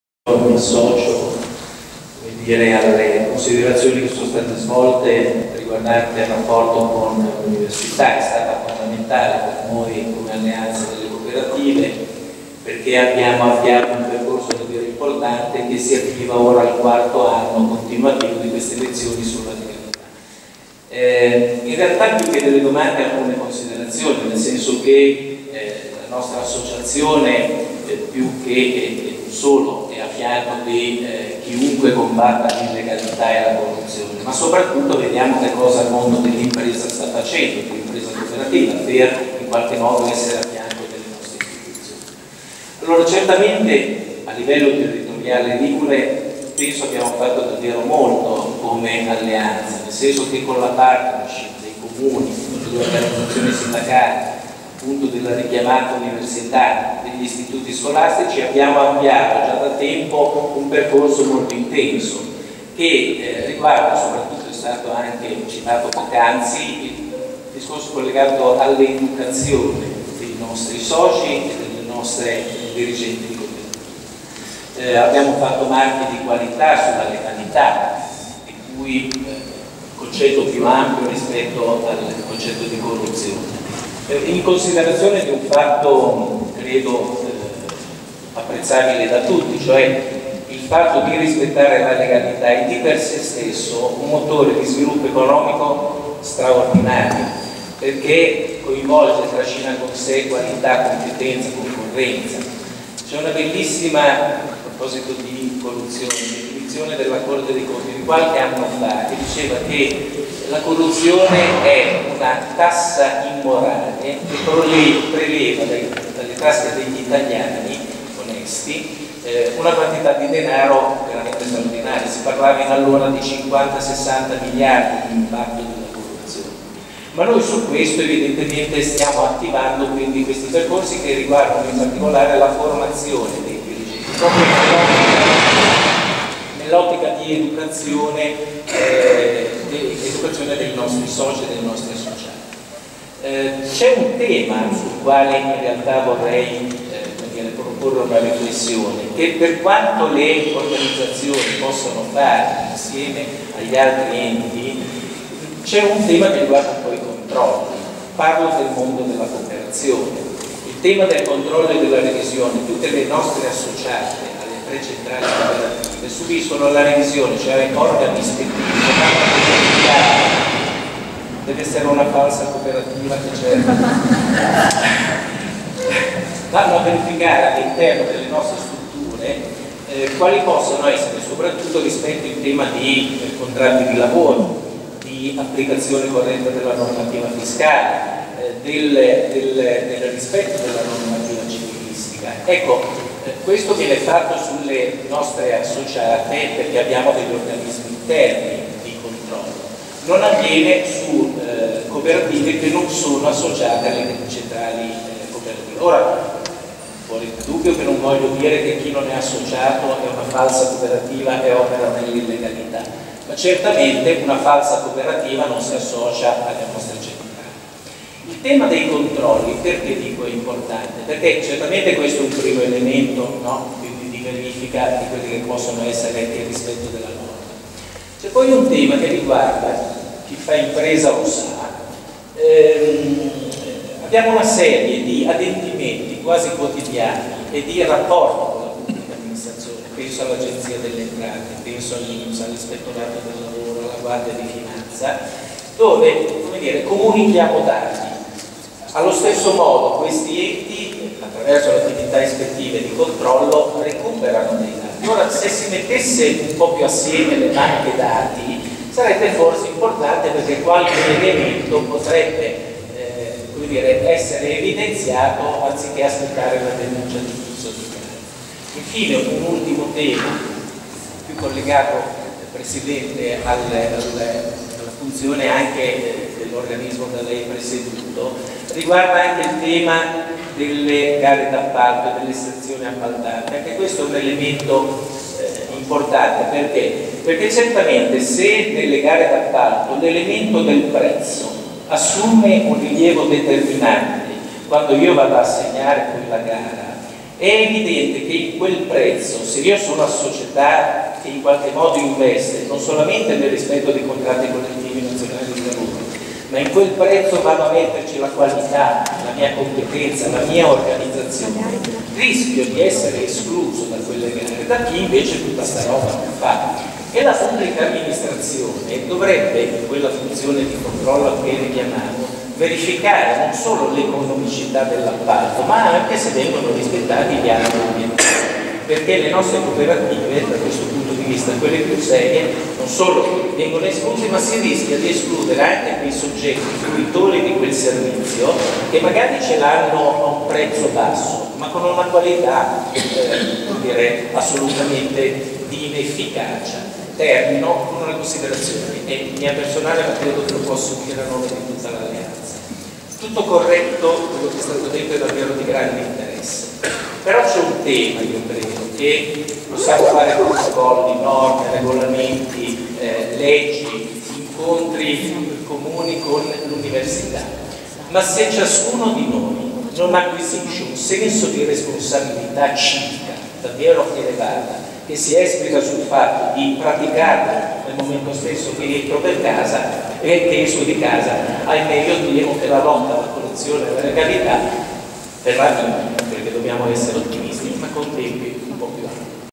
Marastoni " Alleanza delle Cooperative al fianco di tutti quelli che sono contro l'illegalità "
“Il contrasto alla corruzione  tra prevenzione e repressione”
Dibattito con Raffaele Cantone, Presidente dell’Autorità Nazionale Anticorruzione